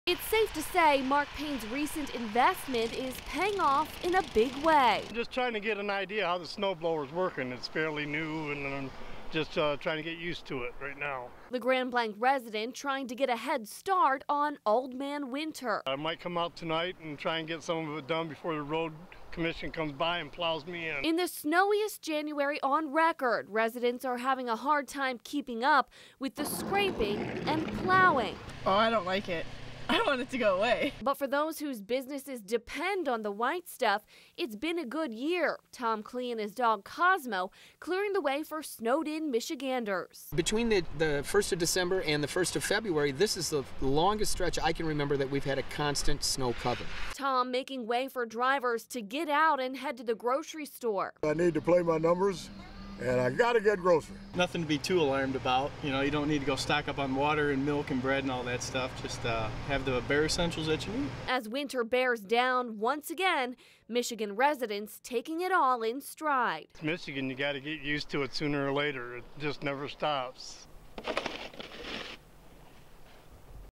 Michigan residents deal with another round of Winter weather
As another significant amount of white blankets Mid-Michigan, some residents are bracing for the storm while others are going on with their daily routines.